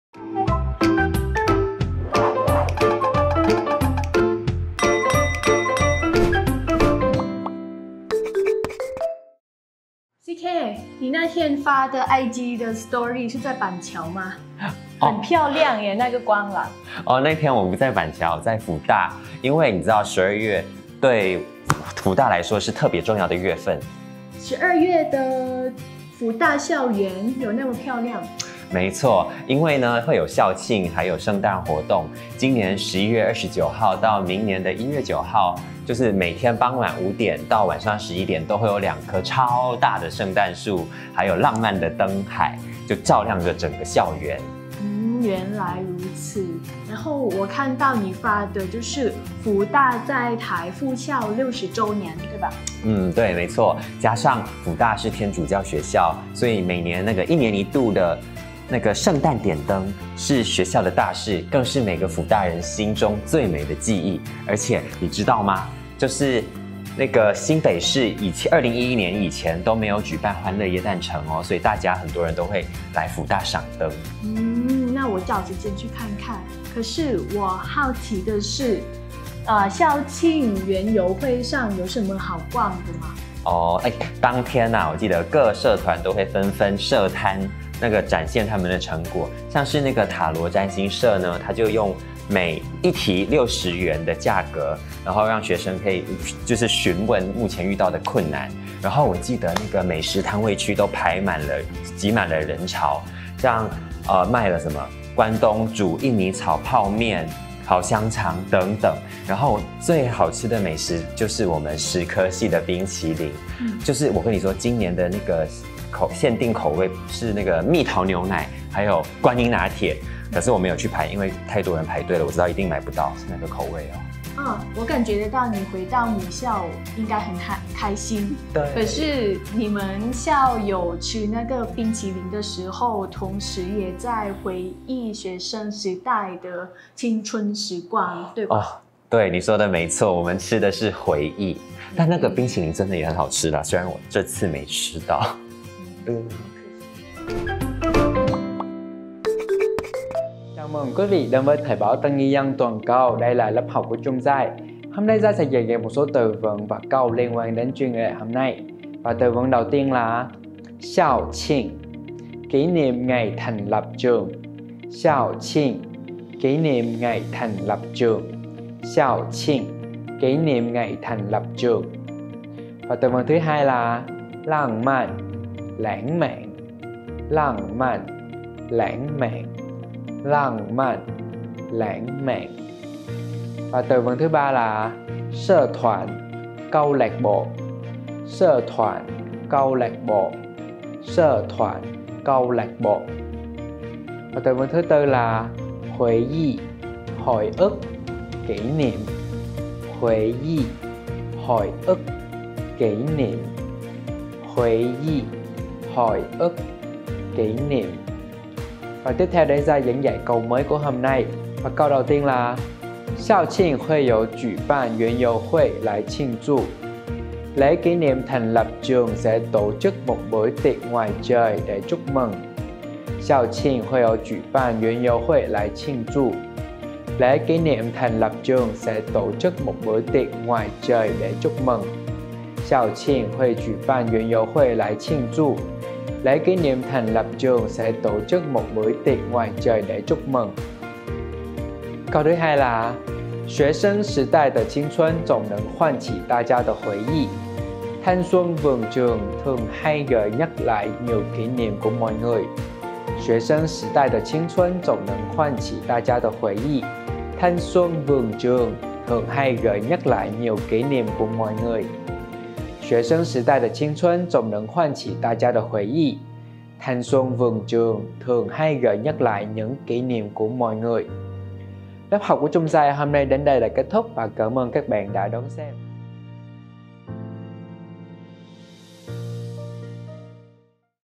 (0102)2/1 – NIA Video học ngôn ngữ: Giáng sinh tại trường Đại học Phụ Nhân
Tháng 12 hàng năm là tháng đặc biệt quan trọng đối với Đại học Công giáo Phụ Nhân (輔仁大學). Thắp đèn Giáng sinh hàng năm là một sự kiện lớn đối với các trường Công giáo, và nhân sự kiện thắp đèn năm nay, Đại học Công giáo Phụ Nhân cũng khai mạc lễ kỷ niệm 60 năm thành trường. Đây cũng là ngôi trường mà CK (Malaysia) - MC của Chương trình bản tin thời sự - Thời báo di dân mới toàn cầu đã từng theo học. Trong tập dạy ngôn ngữ tuần này, CK sẽ chia sẻ với những MC khác về những kỷ niệm khi tham gia lễ thắp đèn Giáng sinh tại Đại học Công giáo Phụ Nhân.